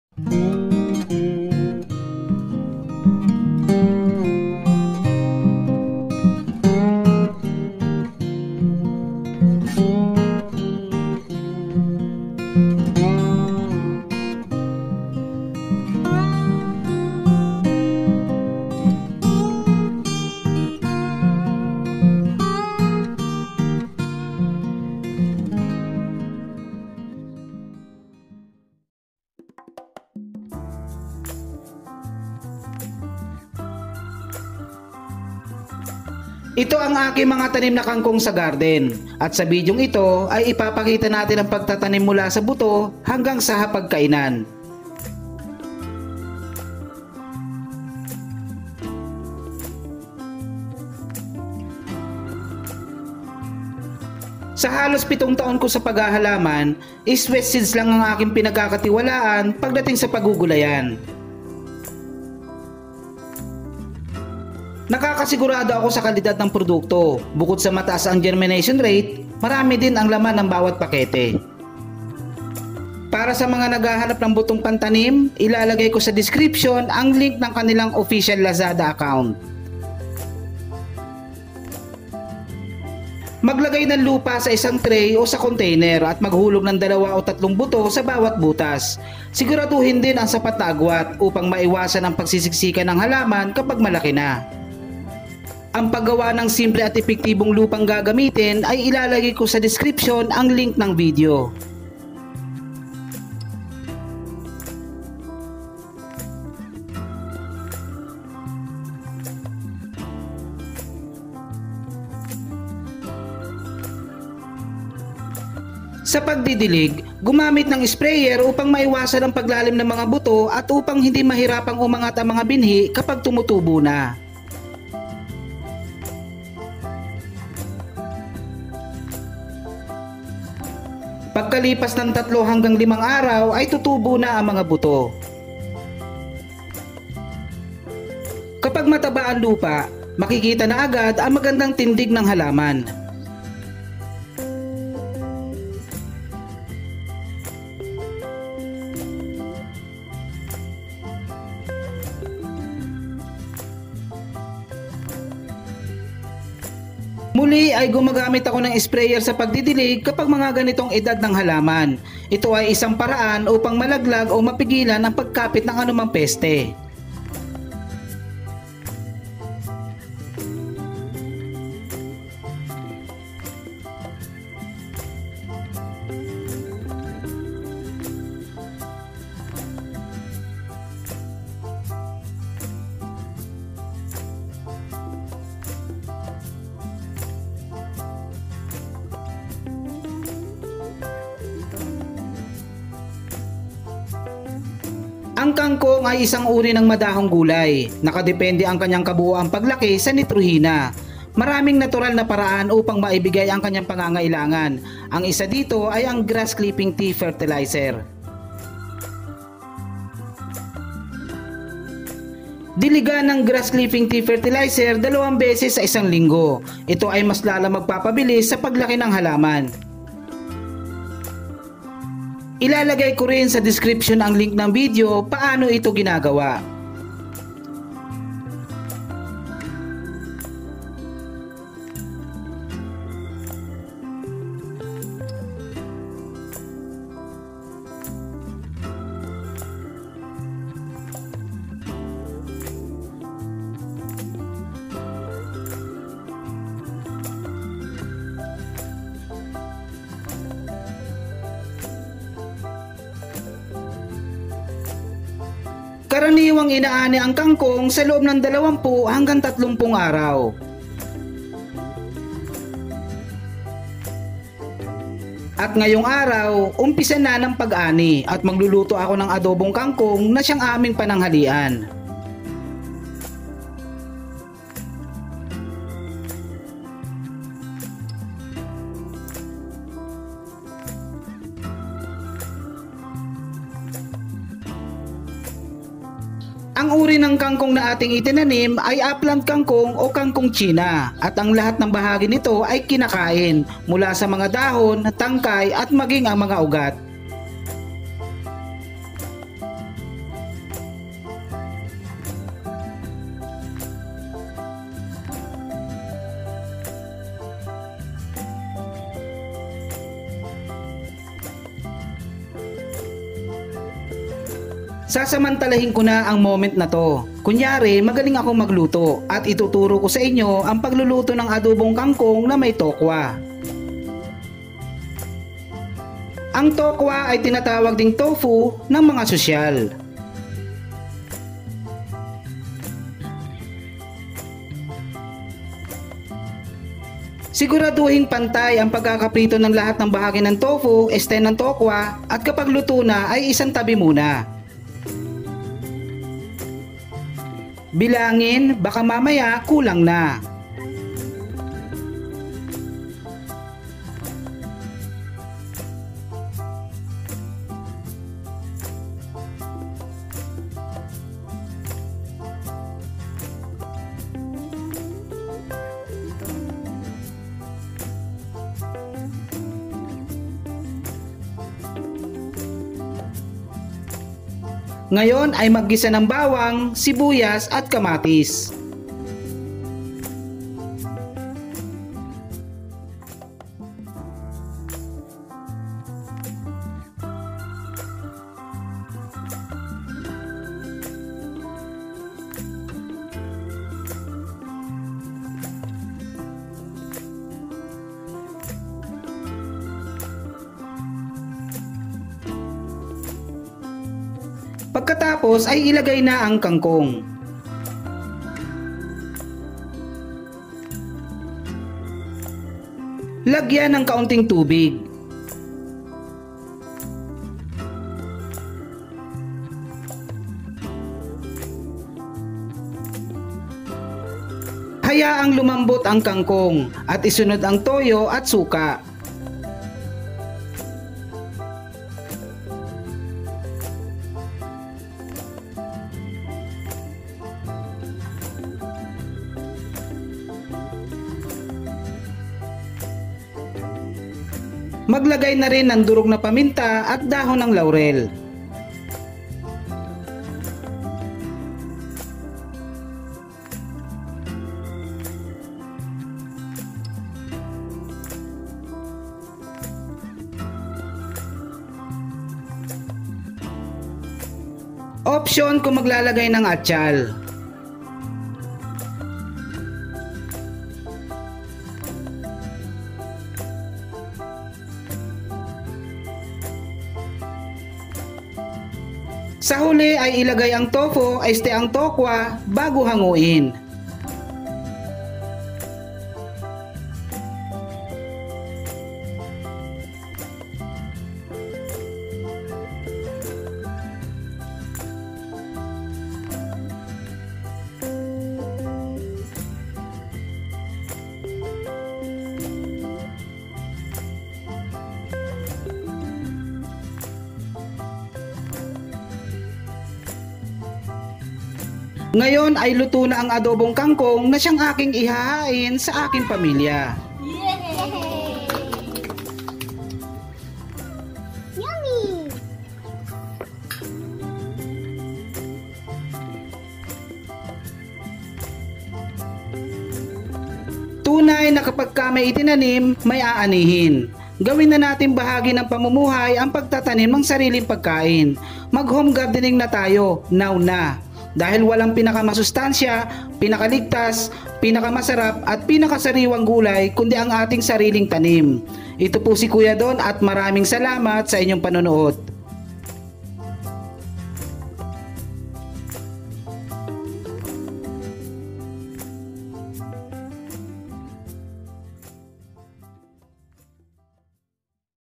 Madaling Paraan Sa Pagtatanim Ng Kangkong
Mula sa pagtatanim ng buto, hanggang maging ulam. Narito ang madaling paraan sa pagtatanim ng Kangkong sa container.